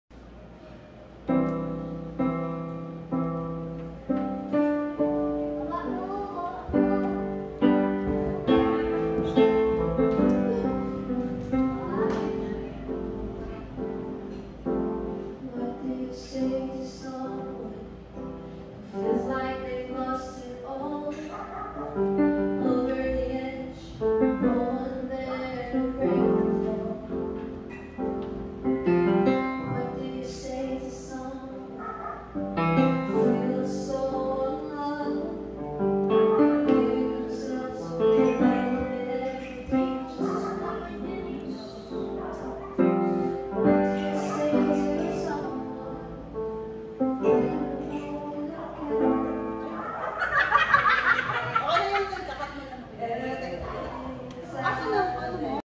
Jimenez siblings bonding
after Church Christmas felowship bonding time with sisters